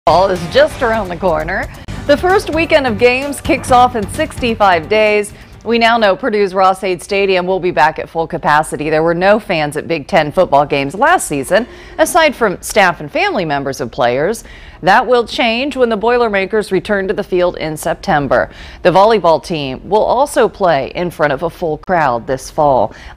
Purdue football announces full capacity
Purdue football announces full capacity at Ross-Ade Stadium.